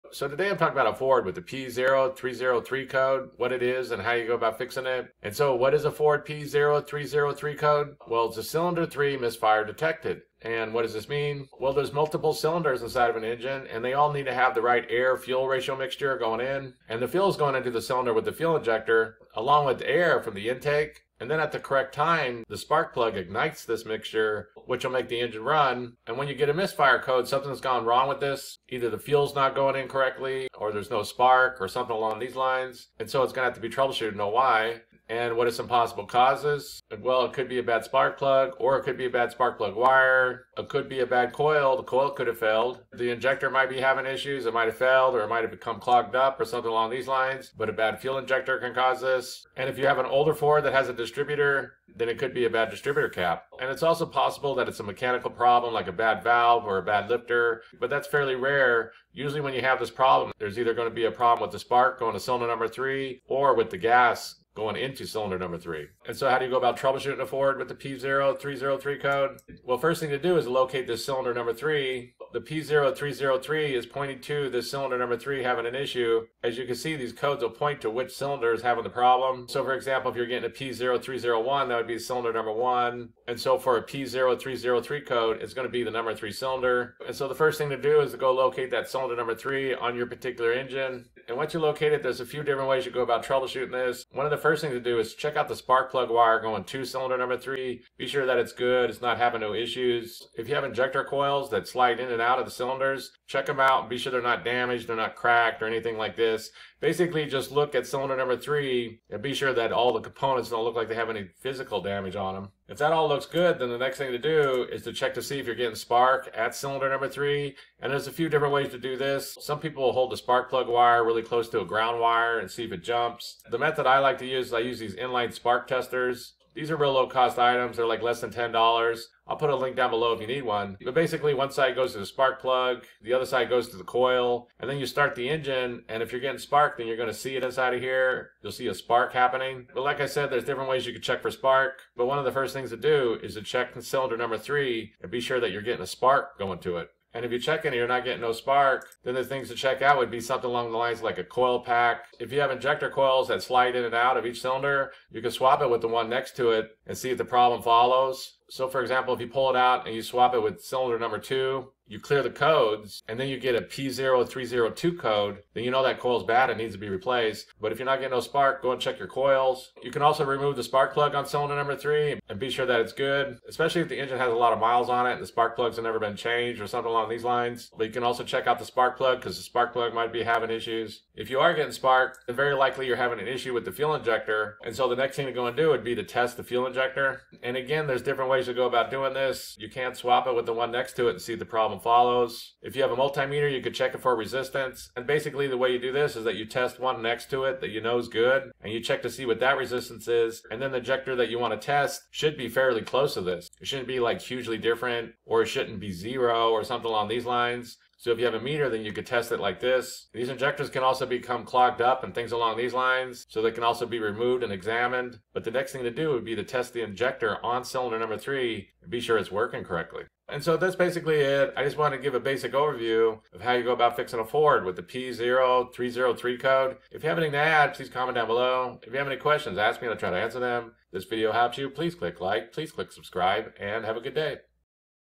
Causes and Fixes Ford P0303 Code: Cylinder 3 Misfire Detected
How to fix a Ford P0303 engine Code: Cylinder 3 Misfire Detected
Spark Inline Tester Tool

Possible Causes of a Ford P0303 Code:
Spark Plug or Spark Plug Wire
Coil
Fuel Injector
Distributor Cap

How to Troubleshoot a Ford P0303 Code:
Locate Cylinder 3
Check for Spark
Check Spark Plug Wires and Coil
Check Spark Plug
Test Fuel Injector